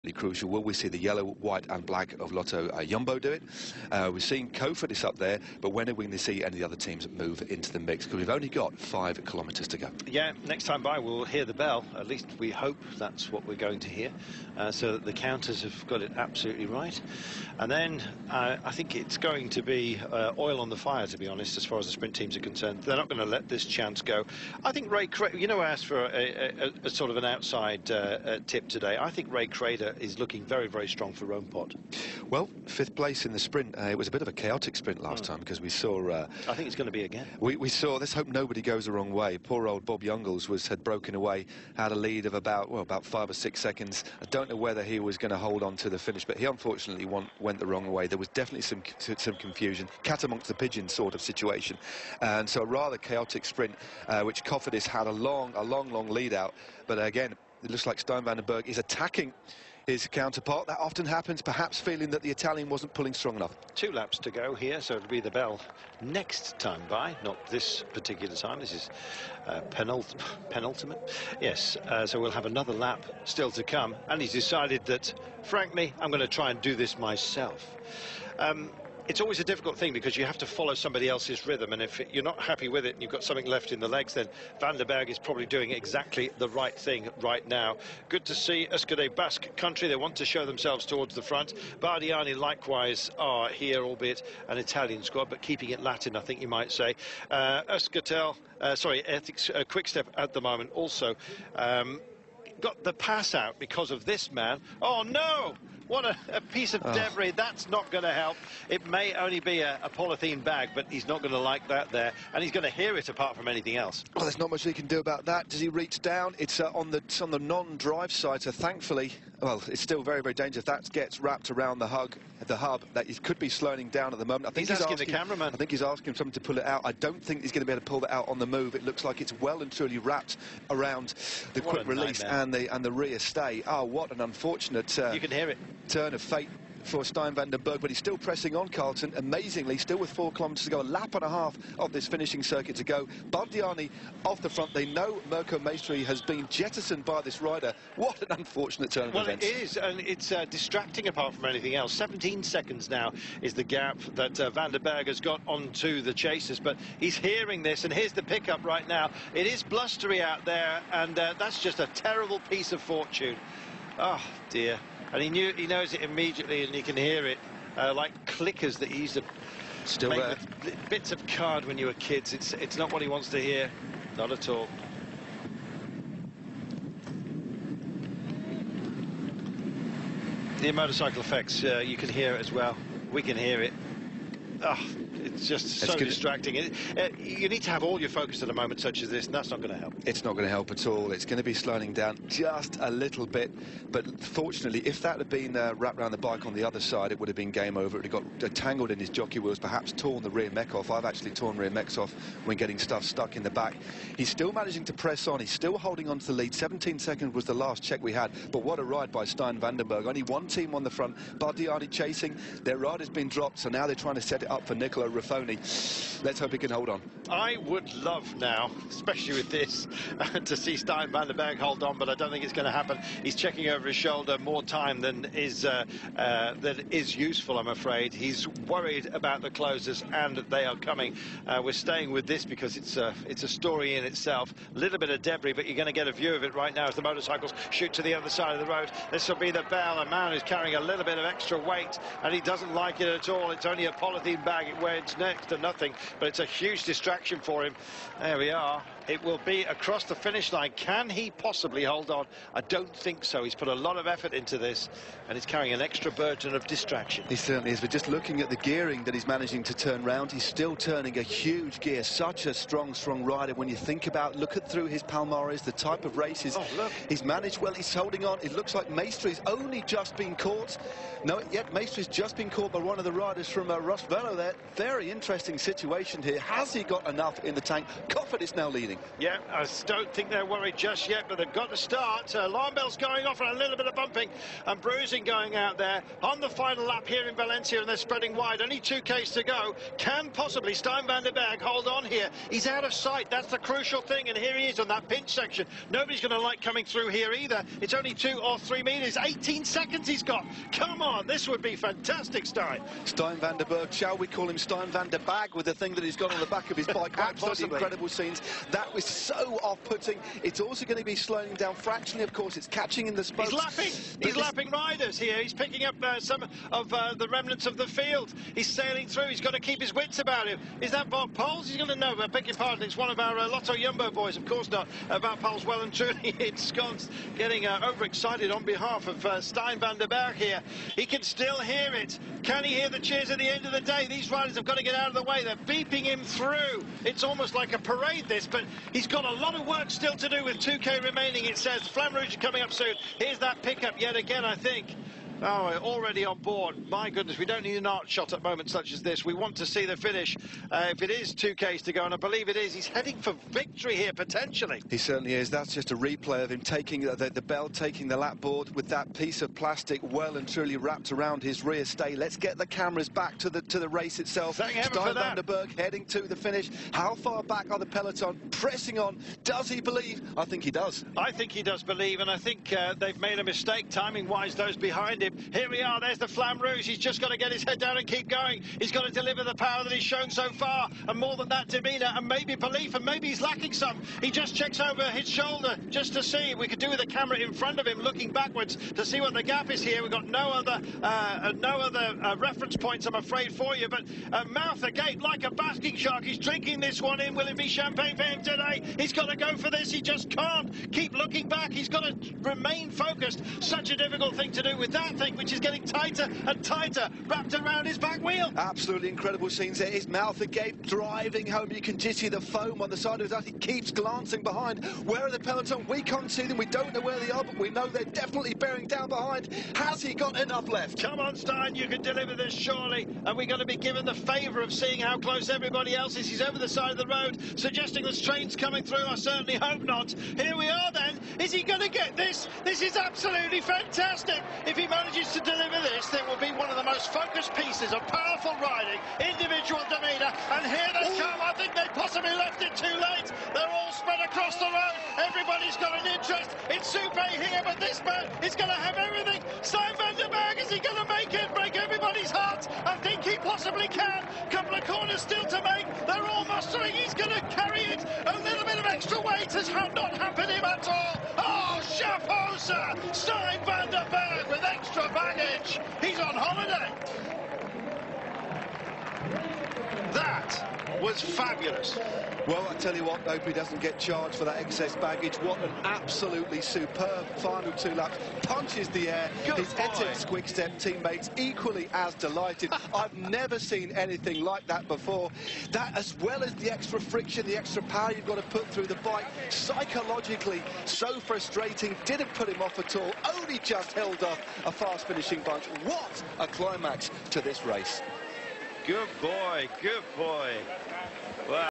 TOUR OF VALENCIA 2016 FINAL STAGE - Stjin Vandenbergh wins with a bag in his wheel
STJIN VANDENBERGH (ETIXX) WINS WITH A BAG IN HIS WHEEL!
TOUR OF VALENCIA 2016 - FINAL STAGE LAST 5 KM (Valencia-Valencia)

Bolsa en rueda Vuelta a la Comunidad Valenciana 2016
Volta a la Comunitat Valenciana 2016